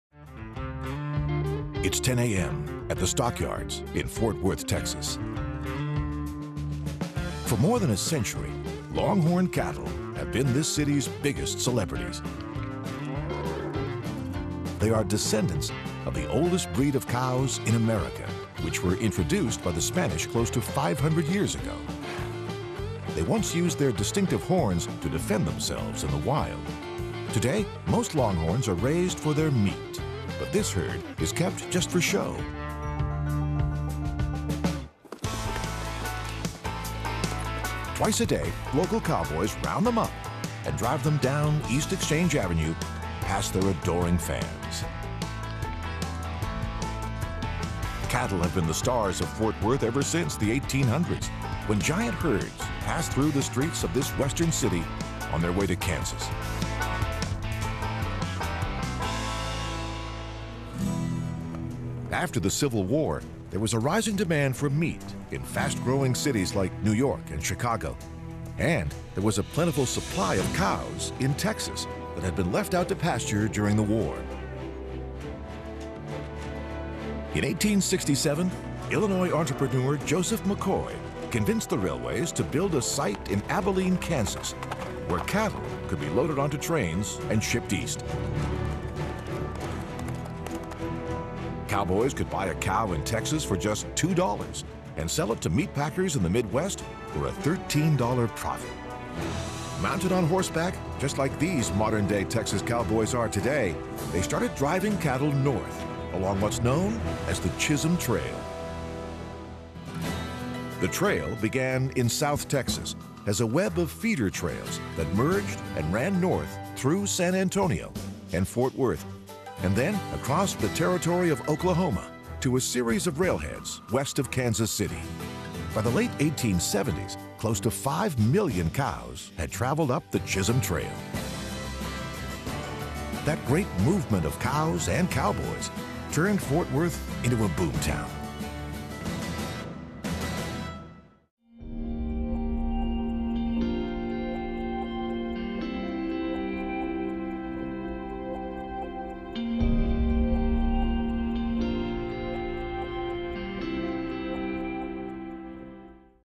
How Longhorn Cattle First Came to Texas
Longhorn cattle are the descendants of the oldest breed of cows in America, introduced by the Spanish. They were also a key element in the development of the Texan economy in the 1800s.